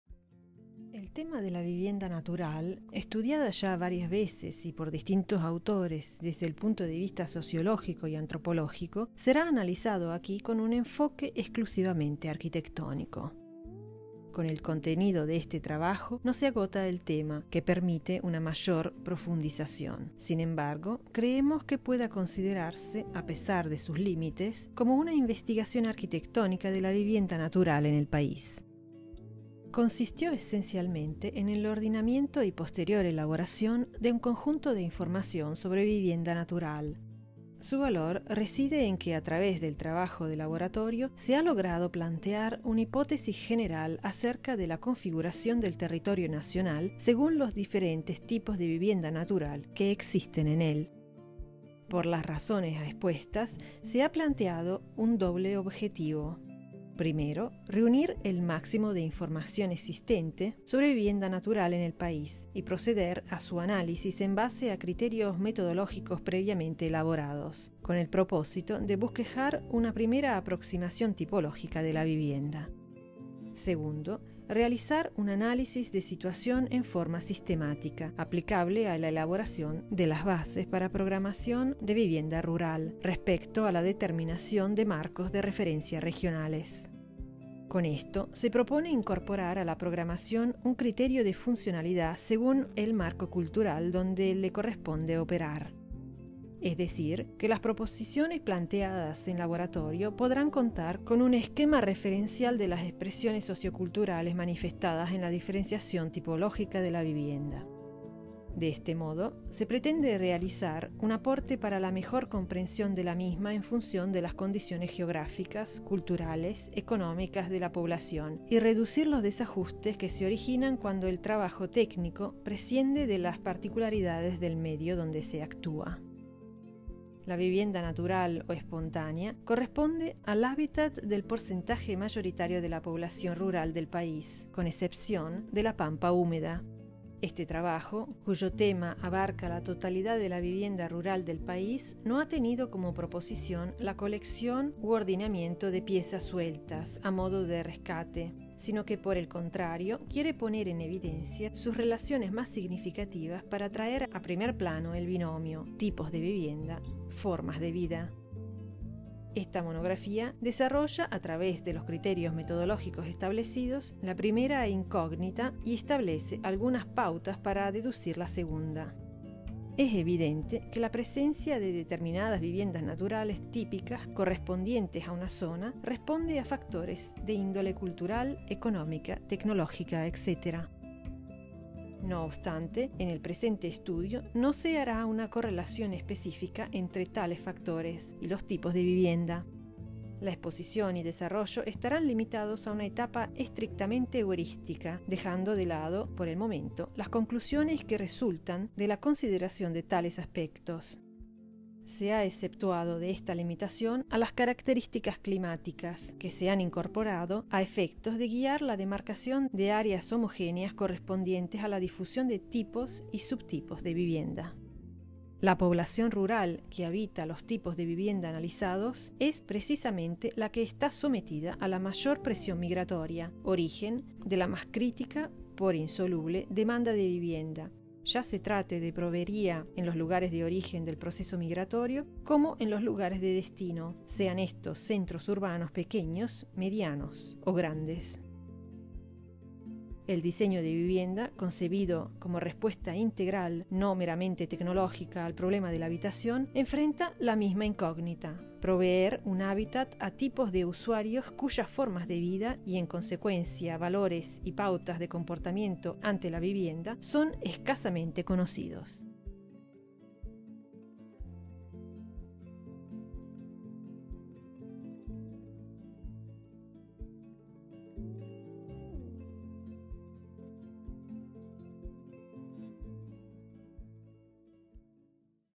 Vernacular architecture in Argentina
Tipos predominantes de vivienda natural en la República Argentina, by Instituto de Investigaciones de la Vivienda, published in 1972 by Editorial Universitaria de Buenos Aires, Universidad Nacional de Buenos Aires